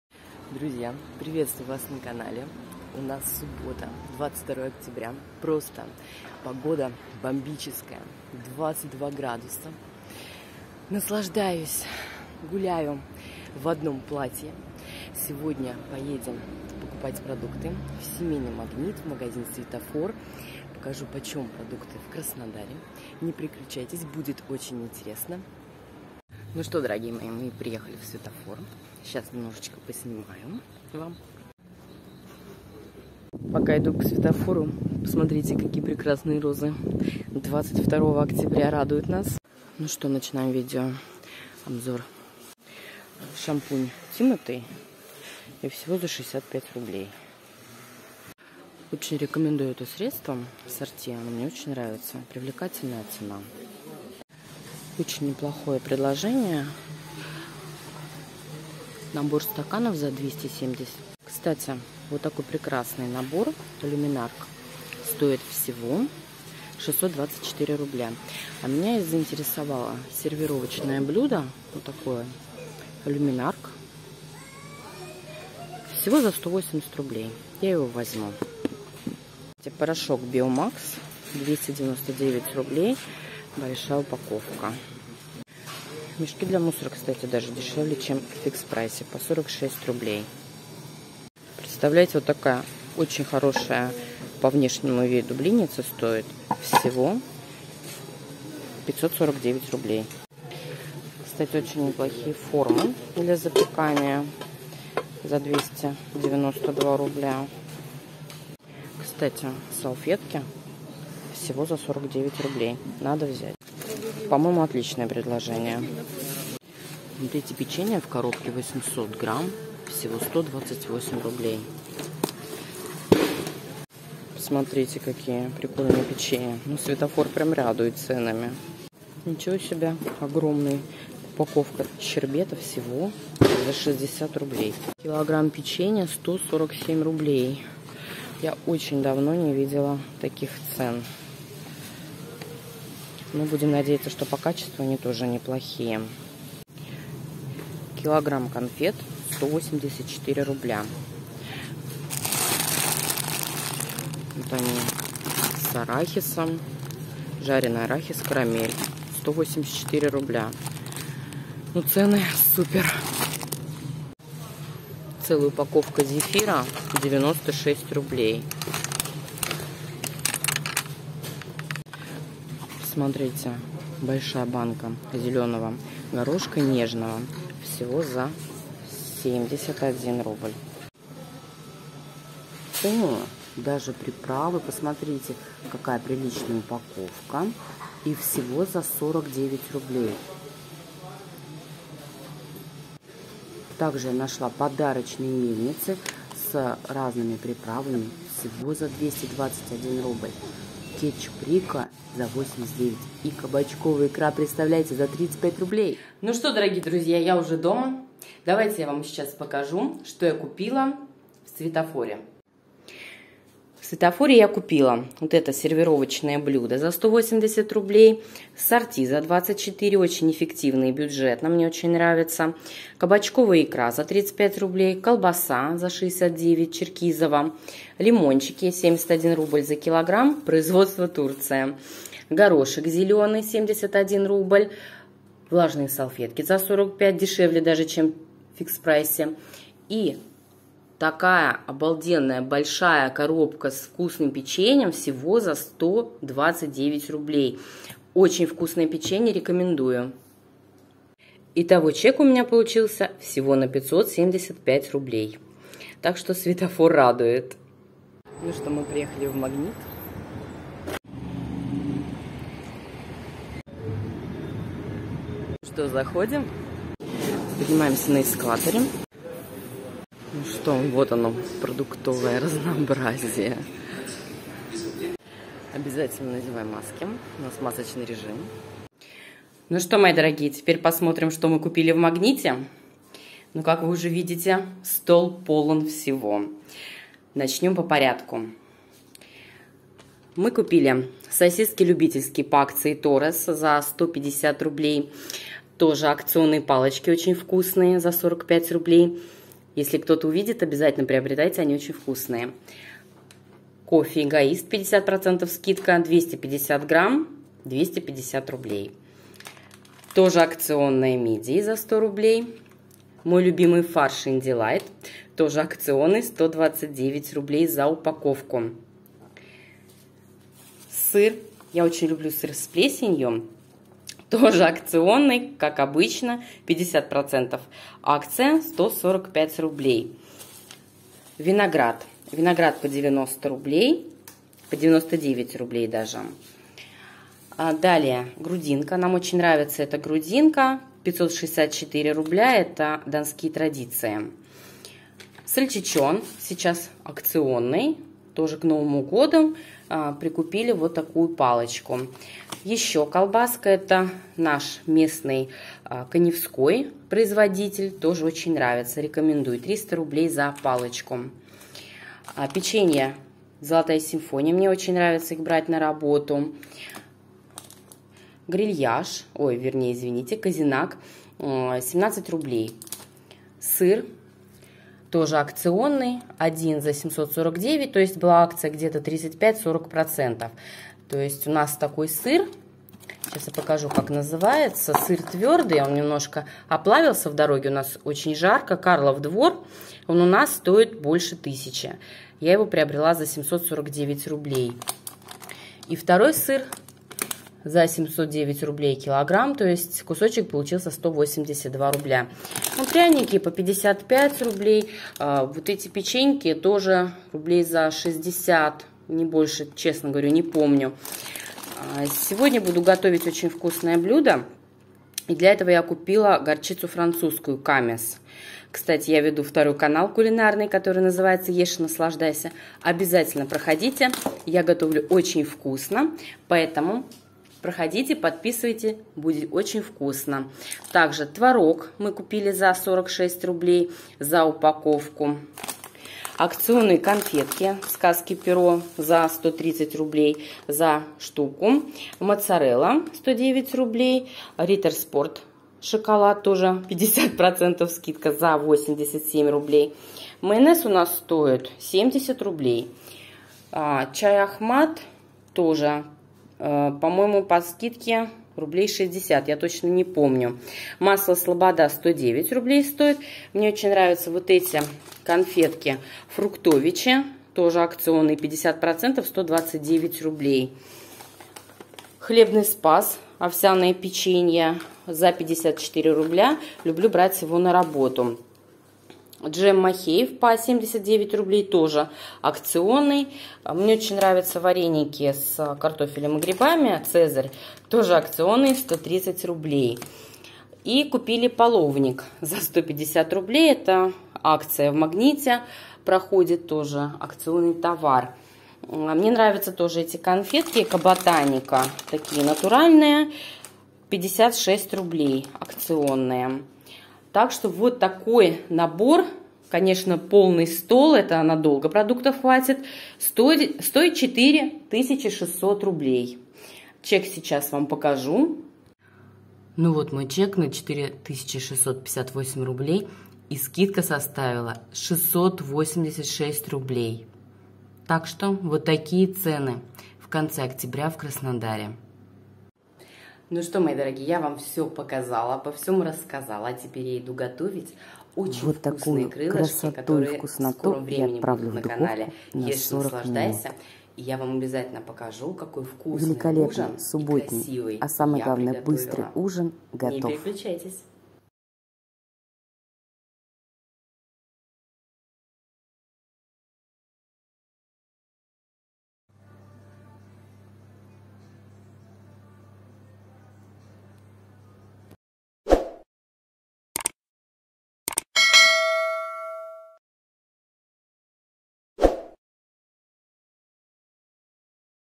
VLOG ОБЗОР ЦЕН В СВЕТОФОР, СЕМ МАГНИТ ЗАКУП
Дорогие друзья! Приветствую Вас на канале о жизни на юге!
В этом влоге я расскажу о своем продуктовом шоппинге в магазинах Светофор и Семейный Магнит и приготовлю очень вкусный и быстрый ужин)))
Посмотрев это видео Вы узнаете о ценах на продукты в Краснодаре в октябре 2021 года.

Ссылка на мой второй кулинарный канал канал  Ешь и Наслаждайся